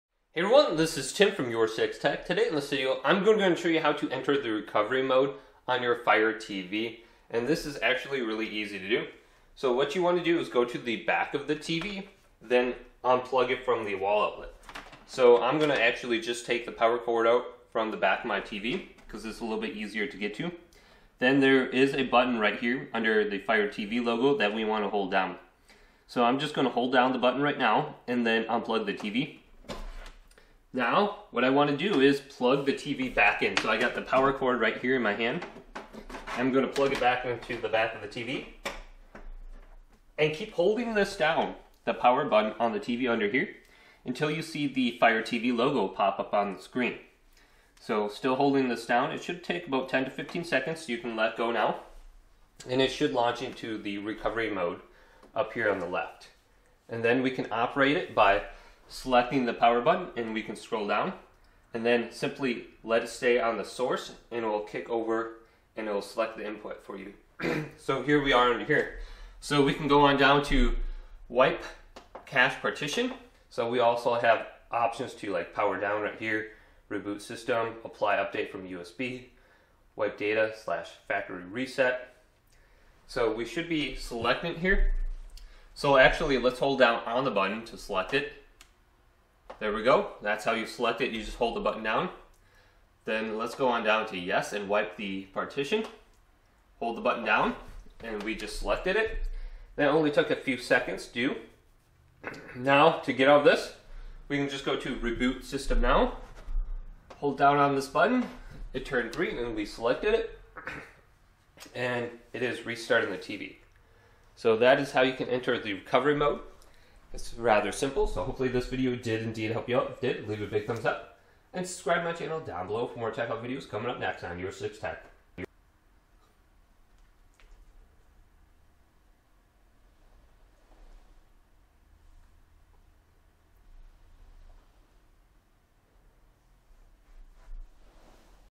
How to Enter Recovery Mode on Amazon FIRE TV (Clear Cache & Reset)
Do you want to know how to enter the recovery mode on Amazon FIRE TV to clear the cache and reset the settings back to default! To do this hold down the power button and unplug the TV from the wall outlet and then plug the tv back in and hold down the power button still and let go until the logo pops up on the screen!